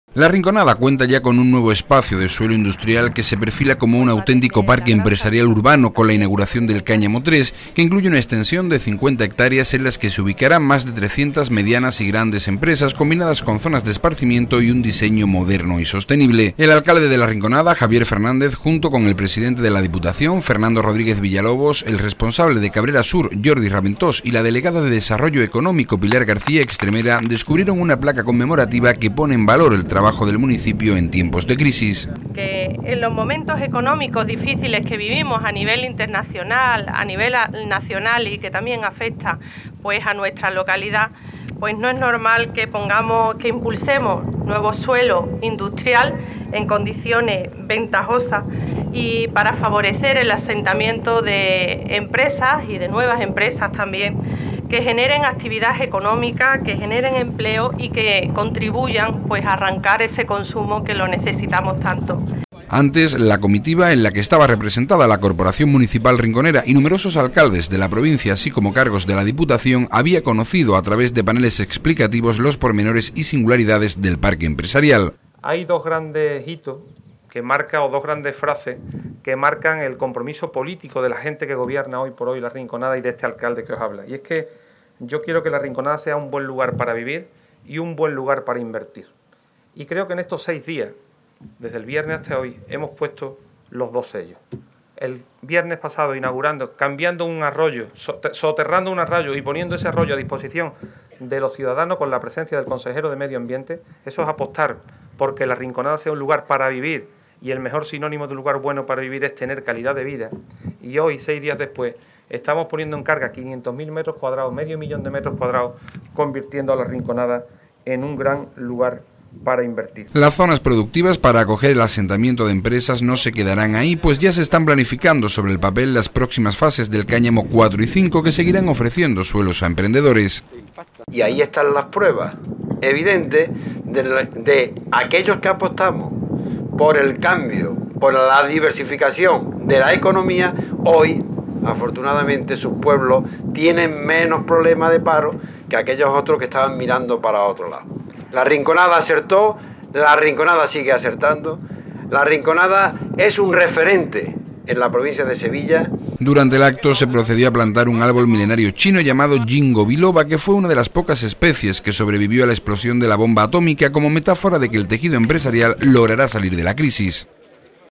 Inauguración del Parque Empresarial Cáñamo III
El presidente de la Diputación, Fernando Rodriguez Villalobos, y el alcalde de La Rinconada, Javier Fernández, inauguran un nuevo espacio de suelo industrial, que se perfila como un auténtico parque empresarial urbano, siendo el único de la localidad que incluye zonas de esparcimiento con un diseño moderno y sostenible. Con una extensión de 50 hectáreas, tiene capacidad para albergar a más de 300 medianas y grandes empresas. La localidad cuenta con 13 parques empresariales en funcionamiento, cuatro de ellos públicos, a los que ahora se incorpora la tercera fase del Cáñamo